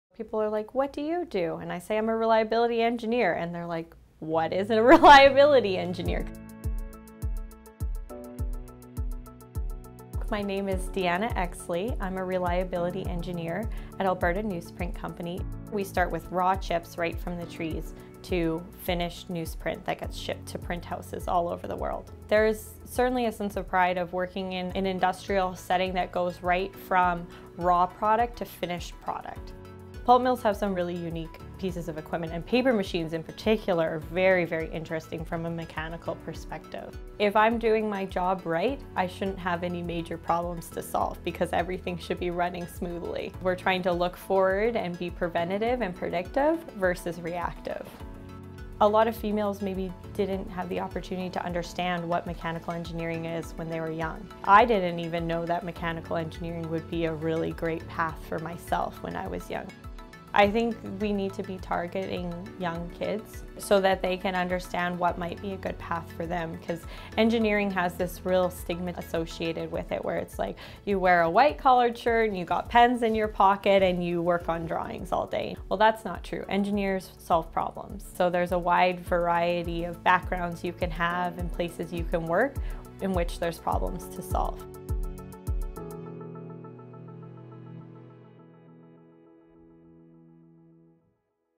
Women in Forestry: Deanna
Deanna Exley, a reliability engineer at Alberta Newsprint Company, explains what the engineering profession really involves.